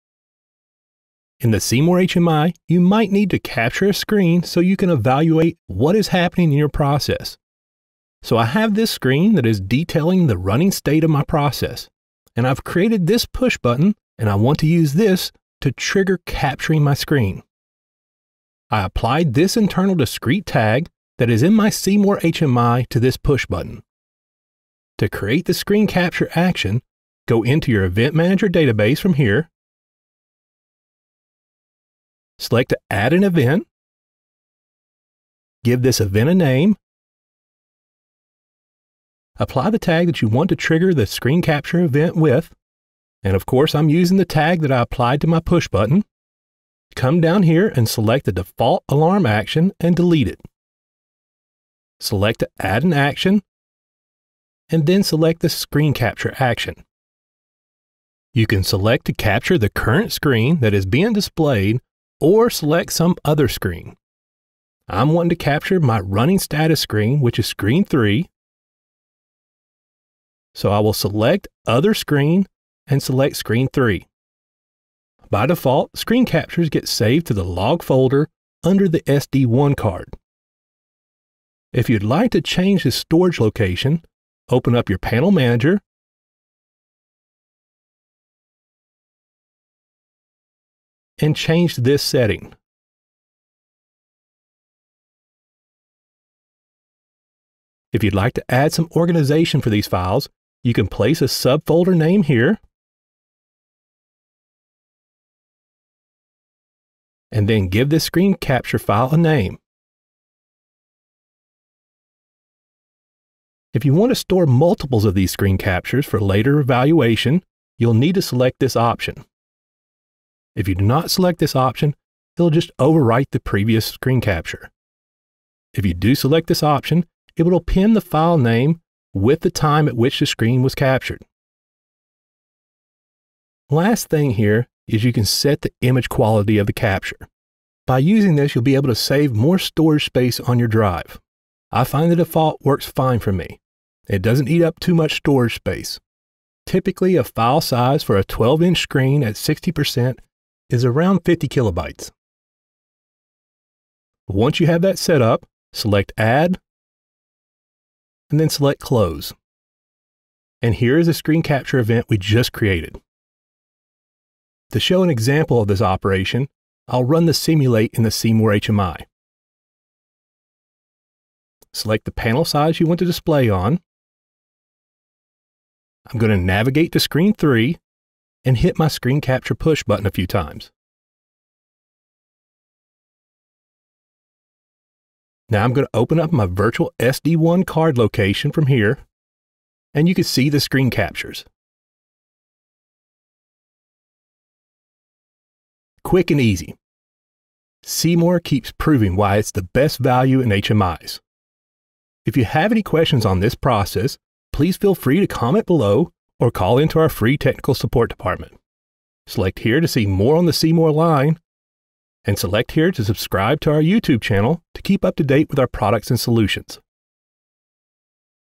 C-more HMI: Collecting Screen Captures from AutomationDirect
- The C-more HMI (human machine interface) is one of the best values in industrial HMI. Not only do you get competitive pricing but the ease of setup and use will save you even more.
In this video, we show how to capture HMI Screen images so that you can evaluate or inform users of issues or improvements to your HMI or control system.
This video is part of the C-more HMI setup videos that give direction on how to set up, use and maintain your industrial HMI.
*** Software version used: 6.52